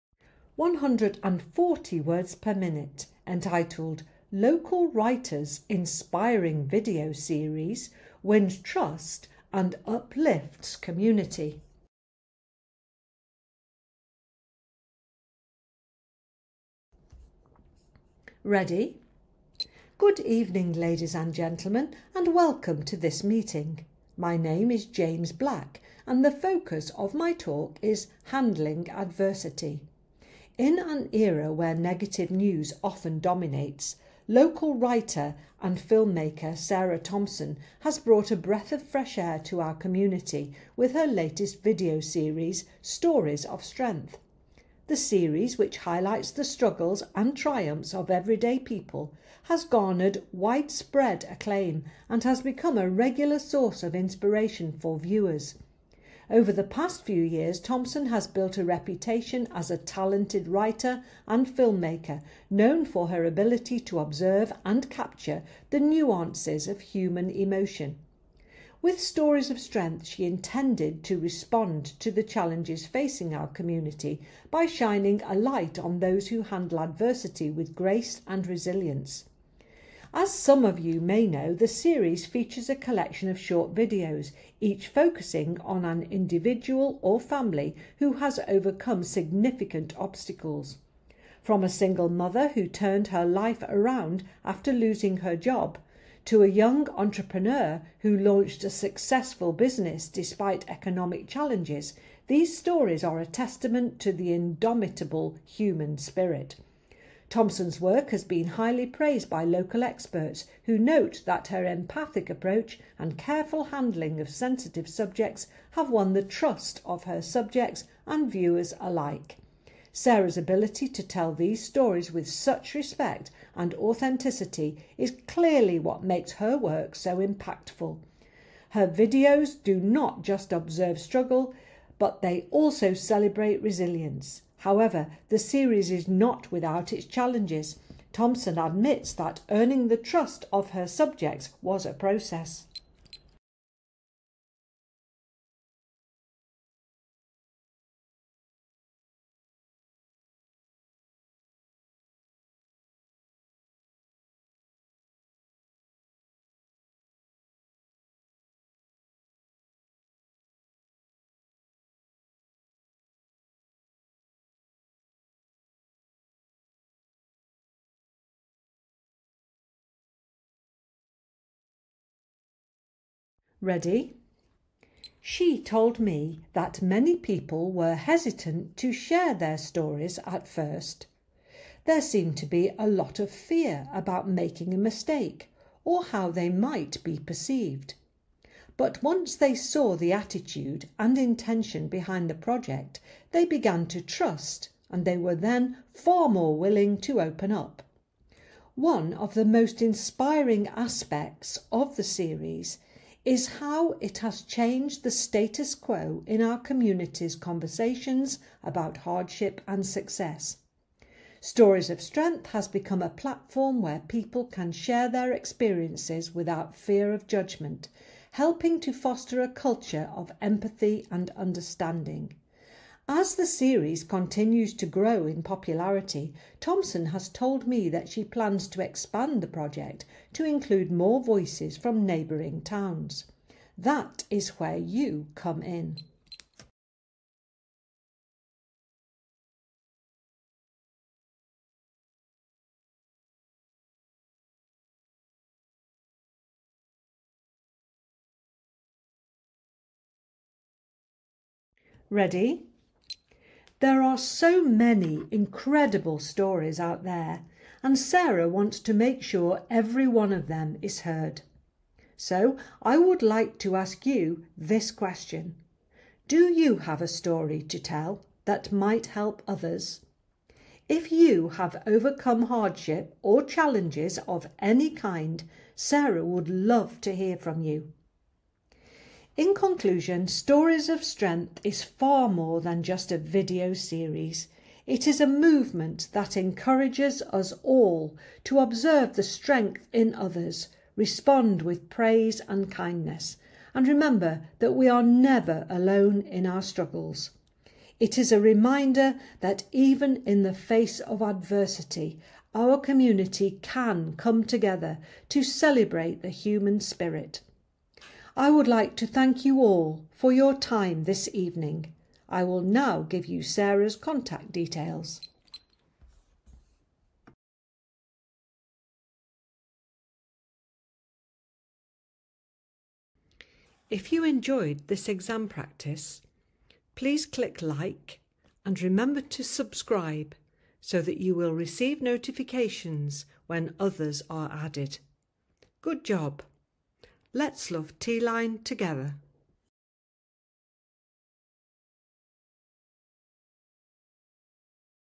140wpm: Local writer's inspiring video series wins trust, and uplifts community
If 140wpm feels too fast, there are still many ways you can utilise this exam practice.  Use it as a reading exercise, another opportunity to identify a quote, learn new outlines or perhaps record alternate sentences.